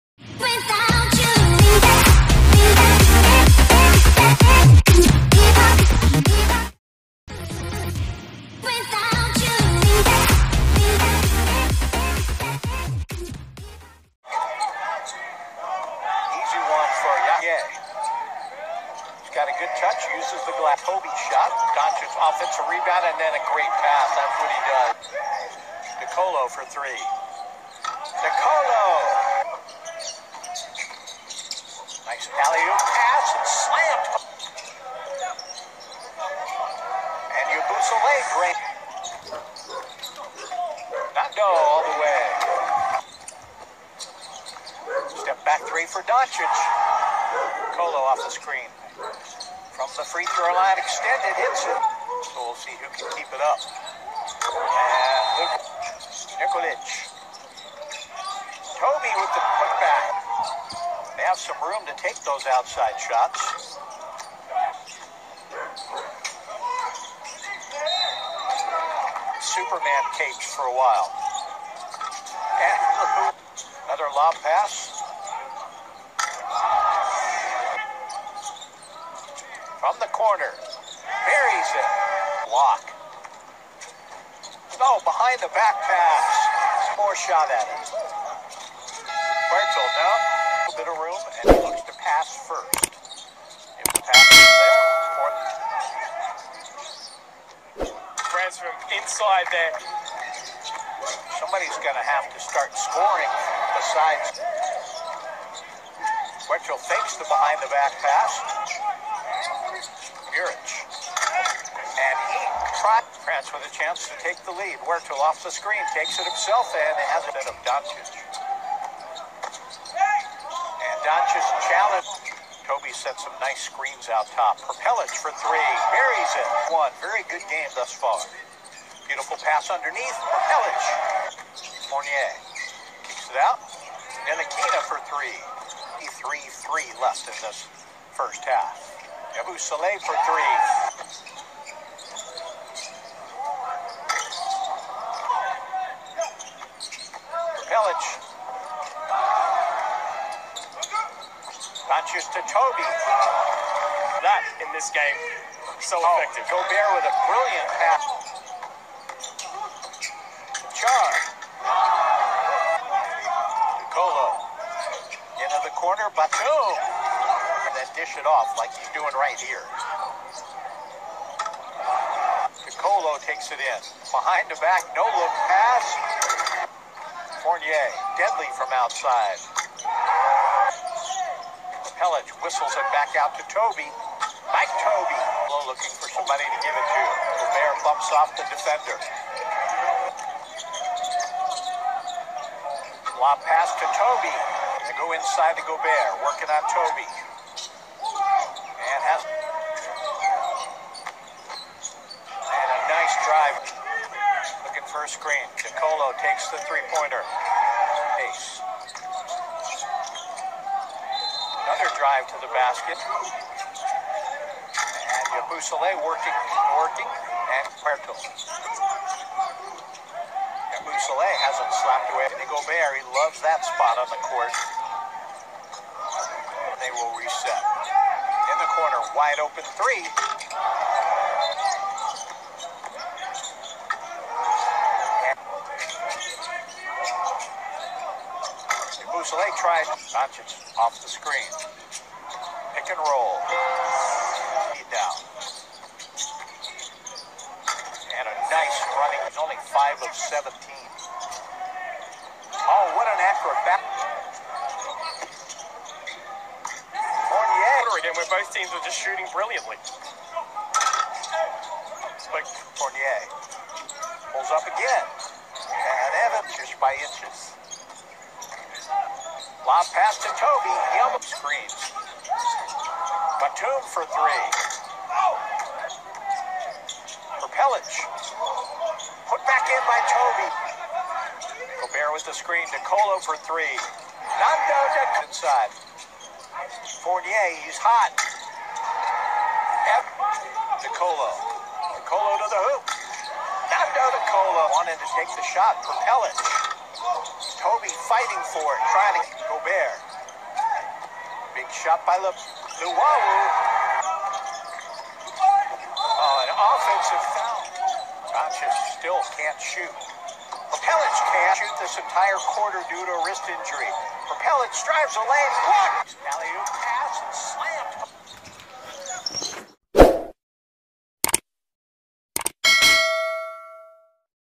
FRANCE VS SLOVENIA SEMI FINALS HIGHLIGTHS | MEN'S BASKETBALL TOKYO OLYMPICS 2020-2021#RjSportstv
Description

FRANCE VS SLOVENIA SEMI FINALS HIGHLIGTHS | MEN'S BASKETBALL TOKYO OLYMPICS 2020